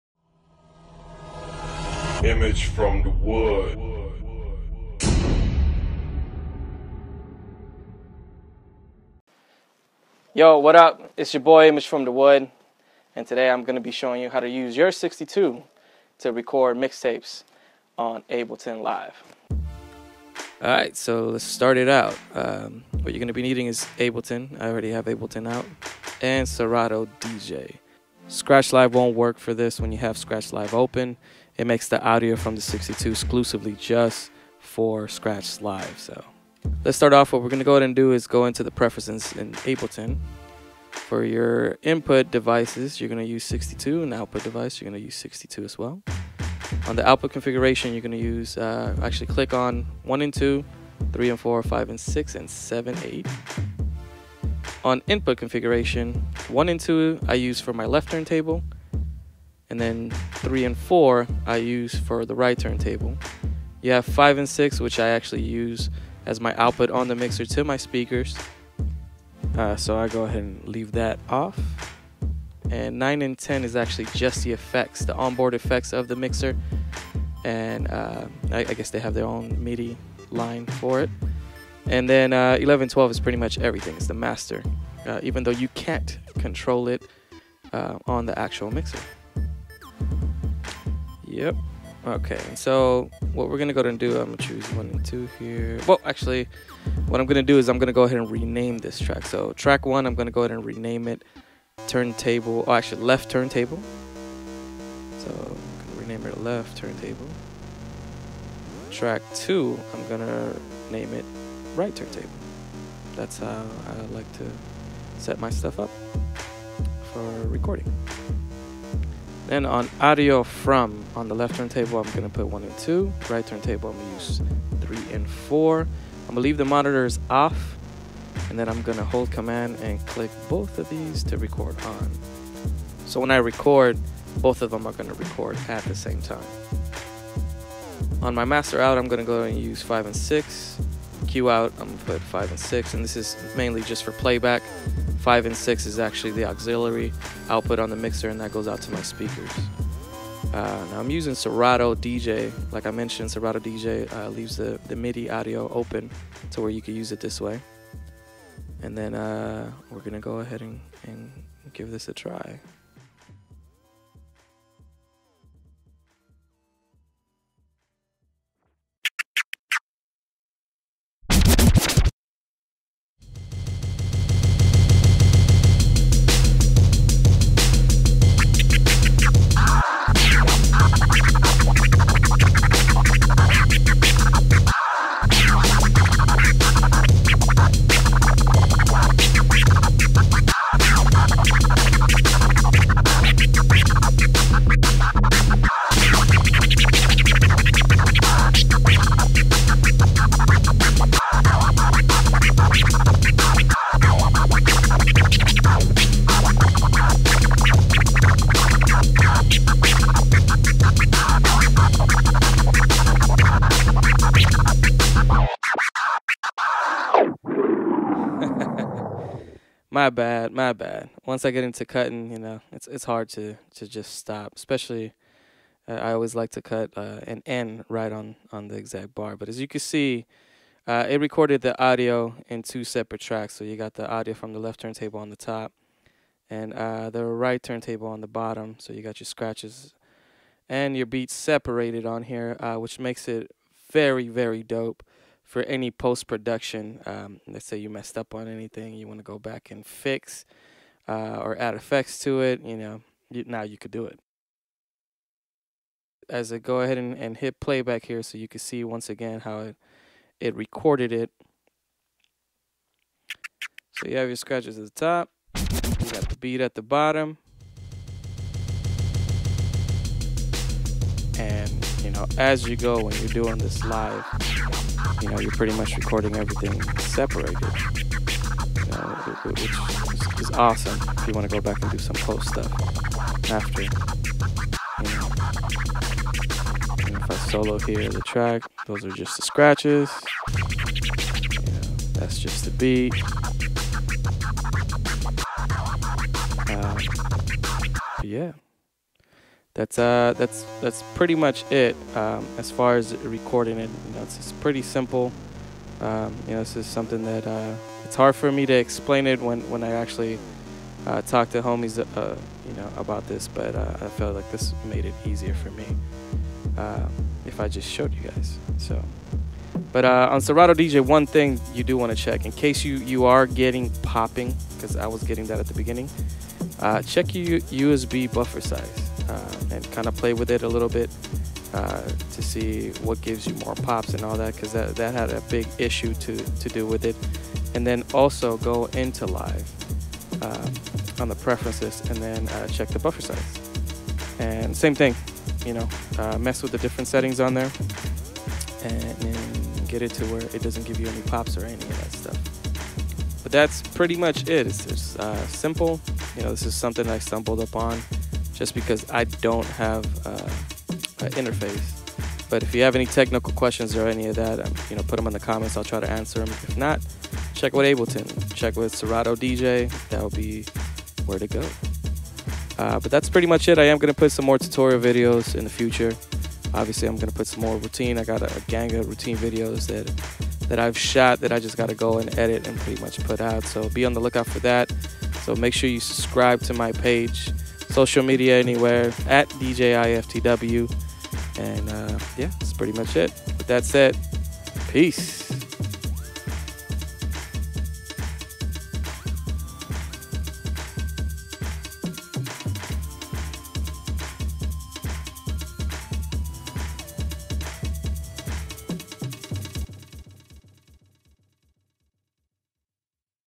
How to use Rane 62 Mixer as an Audio Interface. Mac
Whats up y'all. Today I bring you a tutorial video to show you how you can use your Rane 62 as an audio interface. This is how I have it set up in my studio to record mixtapes and to lay down cuts or samples on production.

I know some of you already knew this or a different way of doing this, but I hadn't seen a video on how to do this so I decided to make one.

I'm running on a Mac, using Serato DJ & Ableton 9.

I apologize in advance if you are on PC or using another mixer or DAW because I don't have any experience doing this with anything else but what I show on the video.